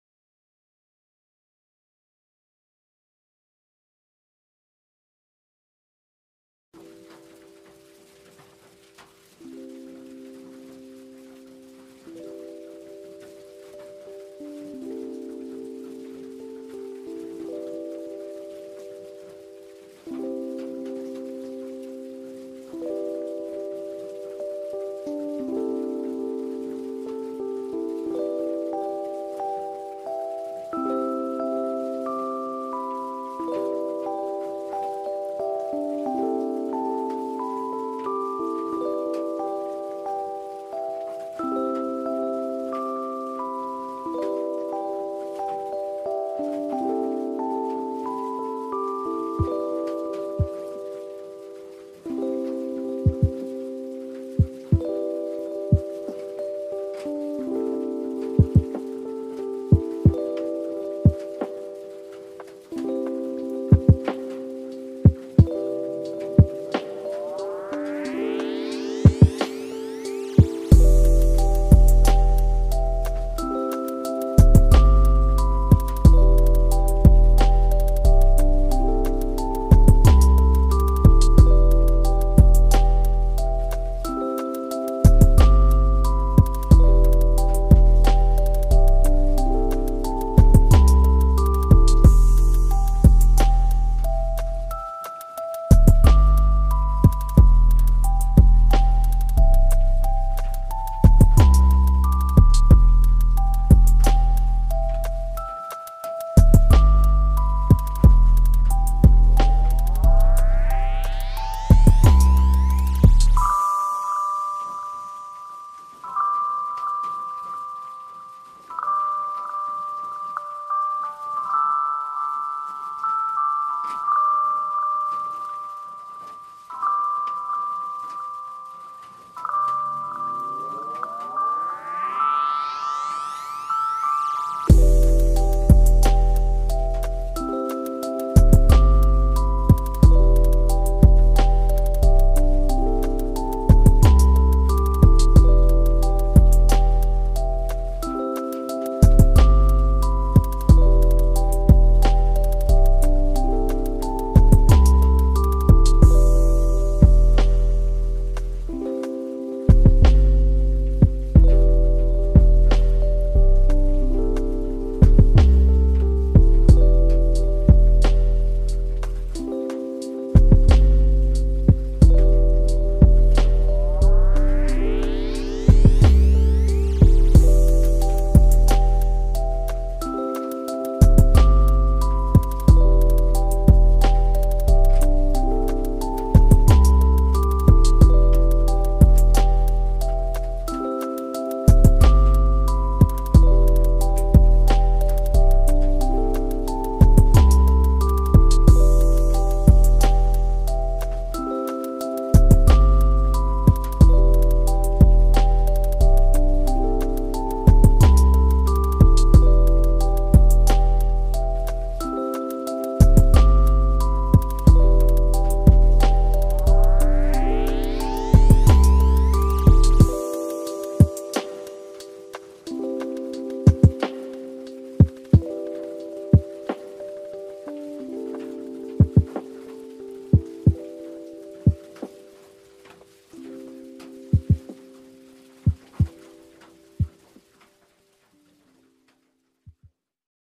( Free for use ) Lofi hip hop beat - Prod. by Mugendz
Let's collab broo, rap on my beat free
This video is about my music instrumental free lofi hip hop rap beat on fl studio mobile if you want to collab with me you can rap or sing on my beat free but give me some credit like :
✓ Put my channel link on your description when you uploaded
✓ Put this video link on description
✓ Write instrumental by Mugendz

Beat no. 82 lofi hip hop beat free

Thankyou